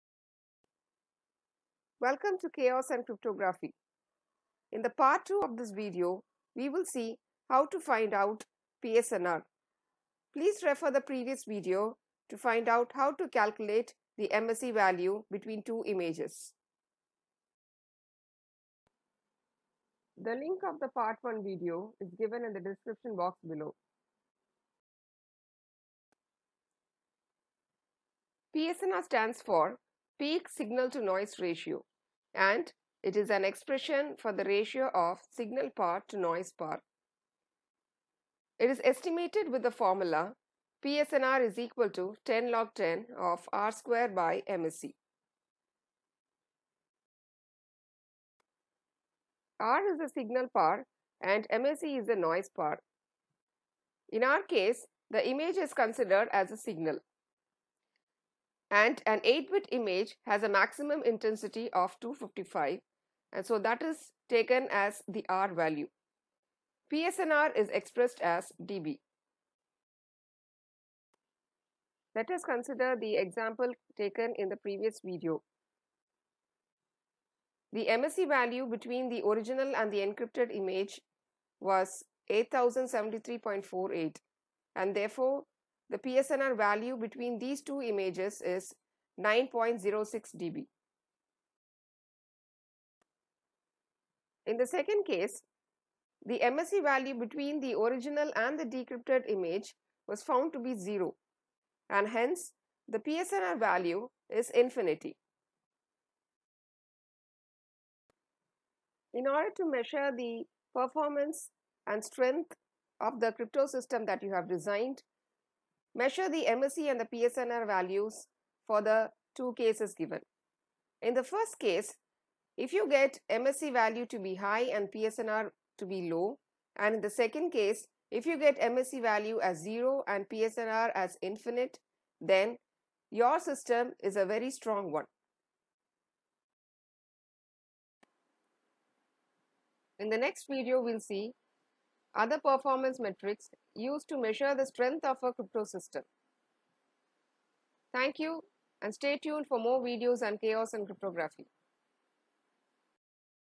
MSE and PSNR - Part 2
This video demonstrates a simple example to understand PSNR

Please refer the Part 1 of this video for better understanding of MSE. The link of this video is given below: